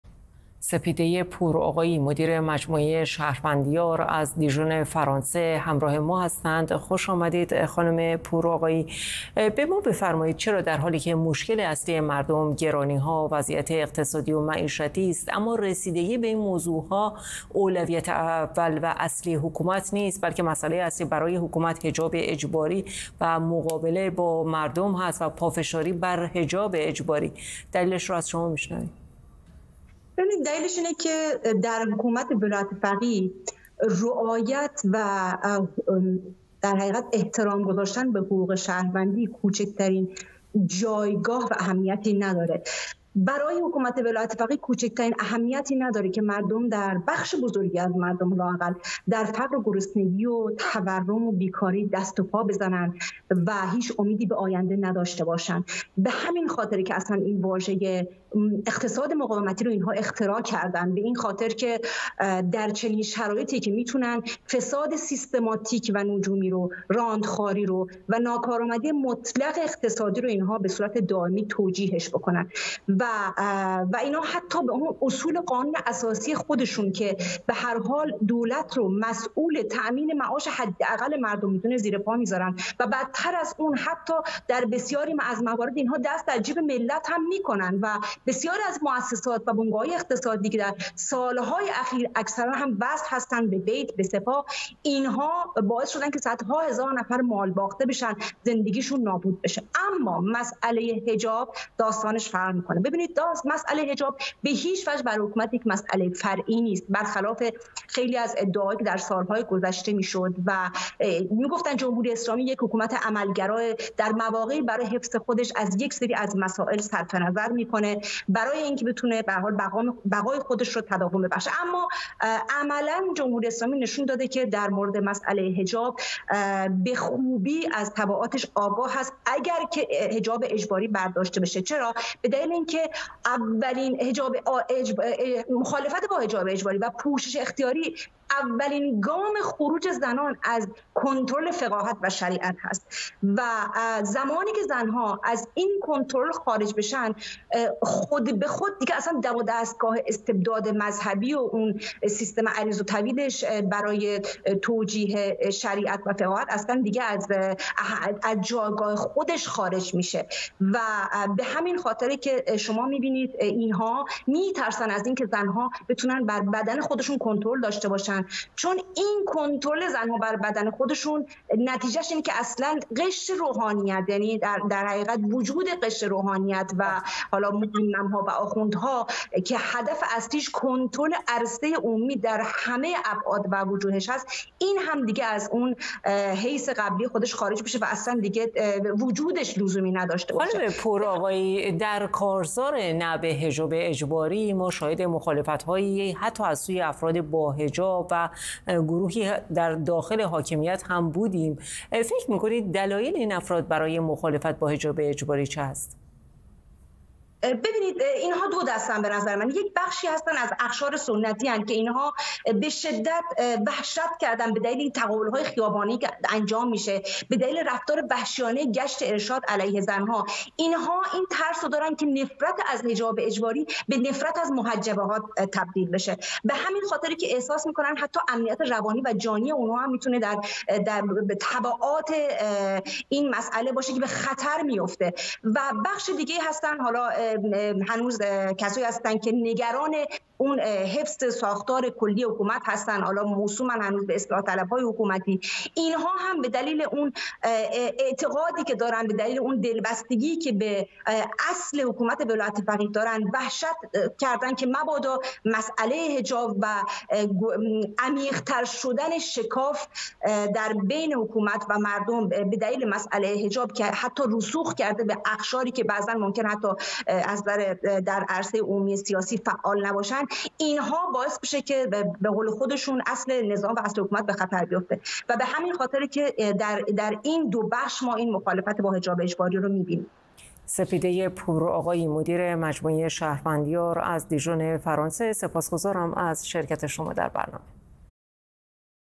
نگرانی حکومت جمهوری اسلامی از استقبال کاربران ایرانی از کارزار نه به حجاب اجباری
گفت‌وگو با سپیده پورآقایی، مدیر مجموعه شهروندیار